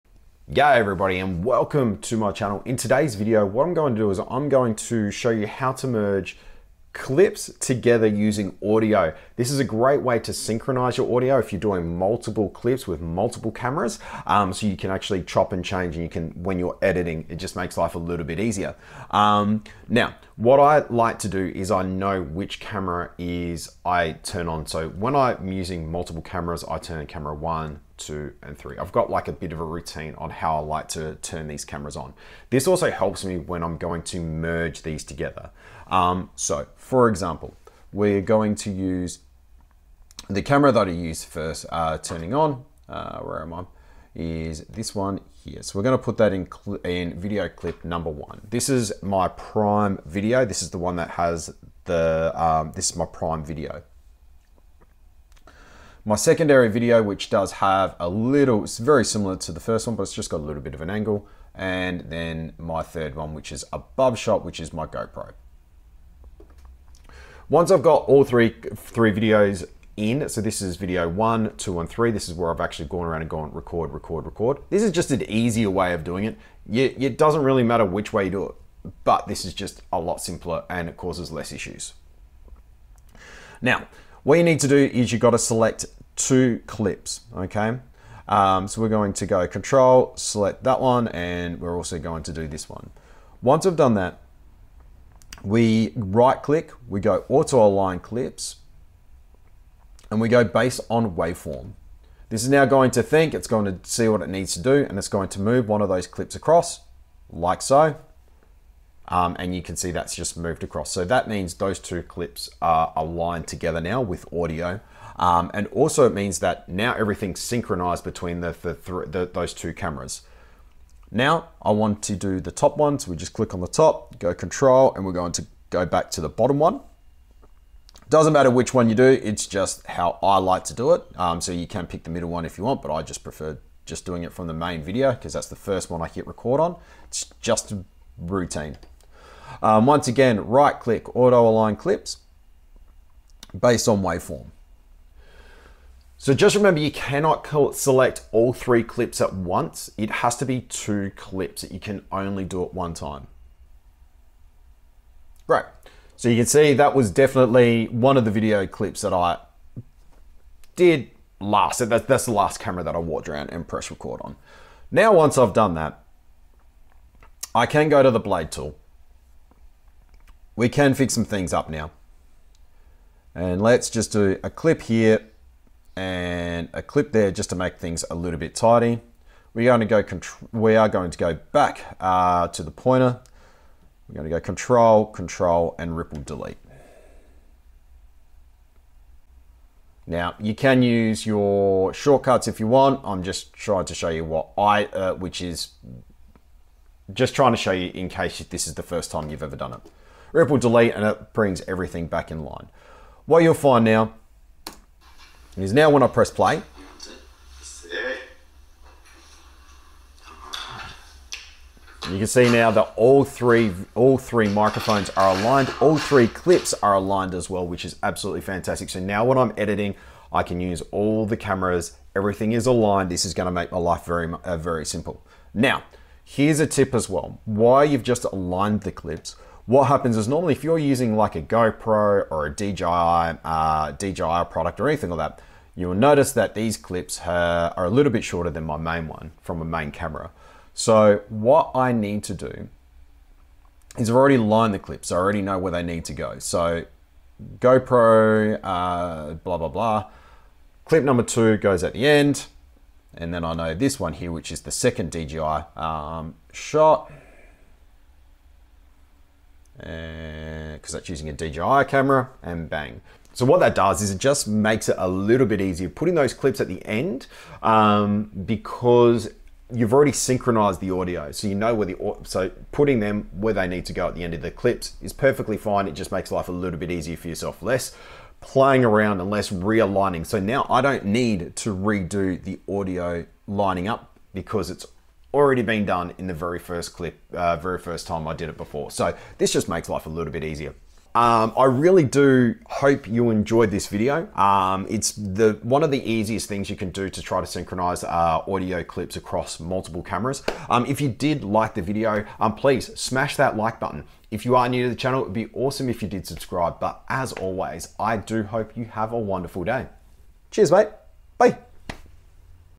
Auto Align Clips in DaVinci Resolve: Even Your Grandma Can Do It
In this video, I will show you how to auto align clips in DaVinci Resolve in just a few minutes. Even if you're not a techie, you can follow along with this easy tutorial.

By the end of this video, you'll be able to:

Understand the basics of auto aligning clips in DaVinci Resolve
Use the auto align clip
Save time and improve the quality of your videos

Click on the play button and learn how to auto align clips in DaVinci Resolve today!

Here are some additional details that you can include in the description:

The video is for beginners.
The video uses DaVinci Resolve 18.